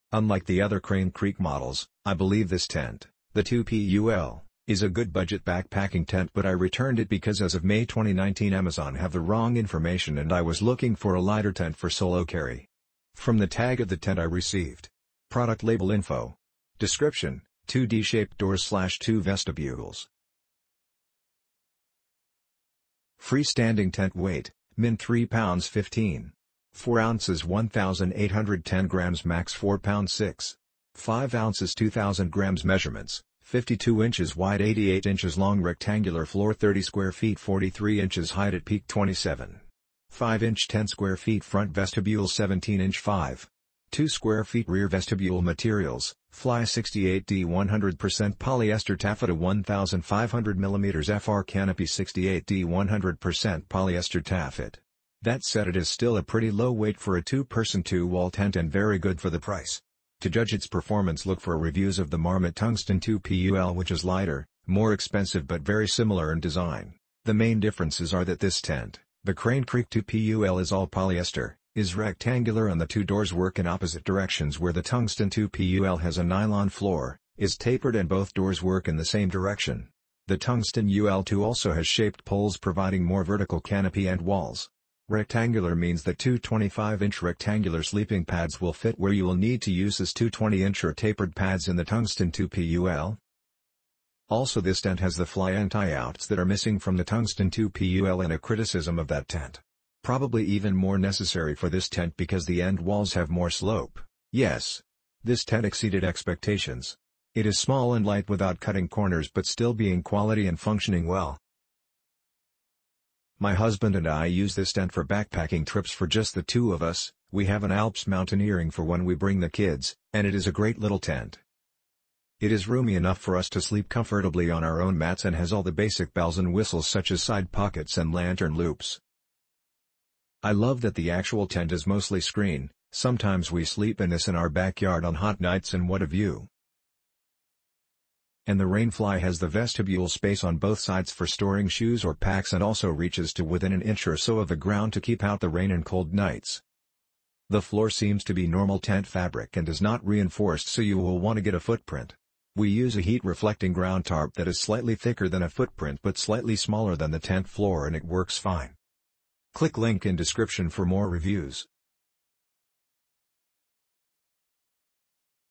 Marmot Crane Creek Backpacking and Camping Tent
Unlike the other Crane Creek models, I believe this tent, the 2P UL, is a good budget back packing tent but I returned it because as of May 2019 Amazon have the wrong information and I was looking for a lighter tent for solo carry.
From the tag of the tent I received... Product Label Info... Description: Two D Shaped Doors/Two Vestibules.
Free Standing Tent Weight: Min 3lb 15.4oz 1810g Max 4lb 6.5oz 2000g Measurements: 52in wide * 88in long rectangular floor 30sqft 43in h at peak 27.5inch 10sqft "front" vestibule 17 inch 5.2sqft "rear" vestibule Materials: Fly 68D 100%...